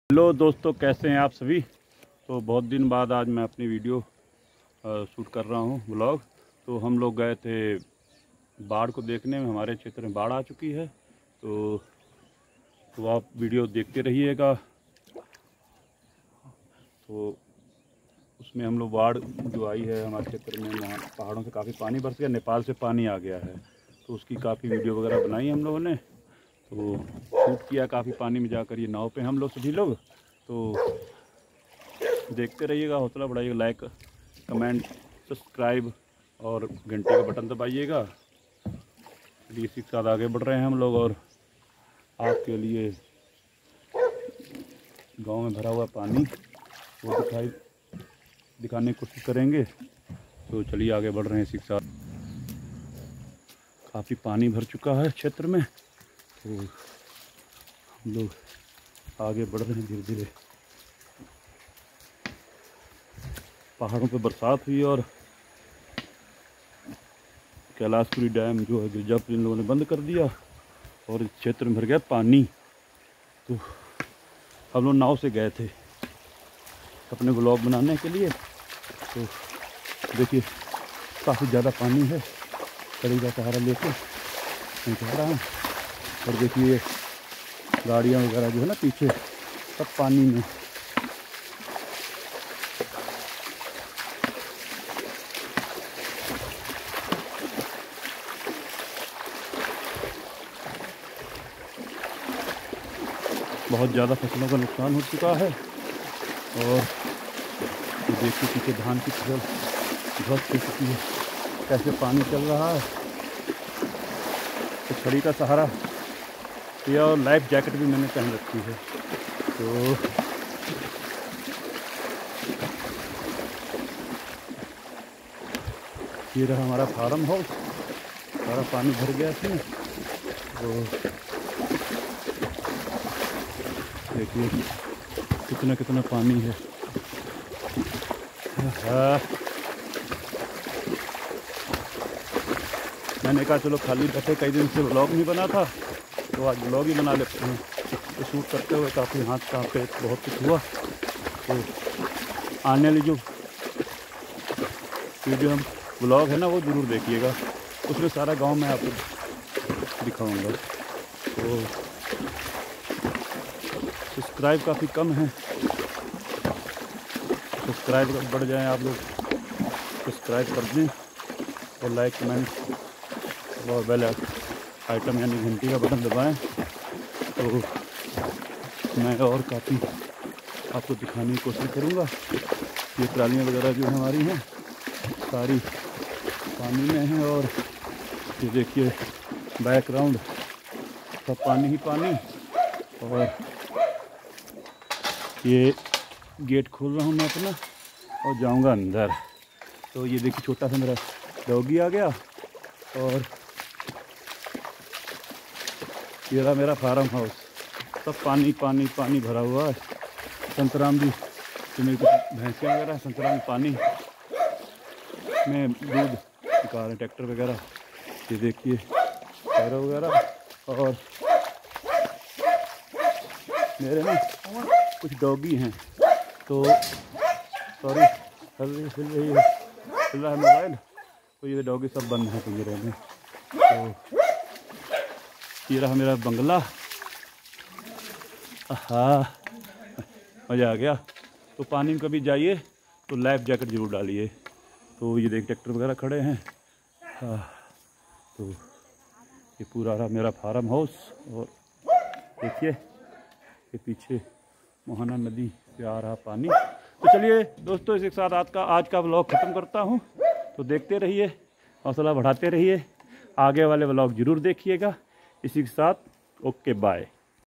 Indo-Nepal Flooded Border , This Killed so many Farmers In last Years
Indo-Nepal Flooded Border ,Distt-Lakhimpur Kheri, Tehsil-Nighashan,Village-Indernagar Near  Banbirpur (Village of Union State Minister Of Home Affairs) (भारत नेपाल बार्डर )मोहाना नदी से भरा बाढ़ का पानी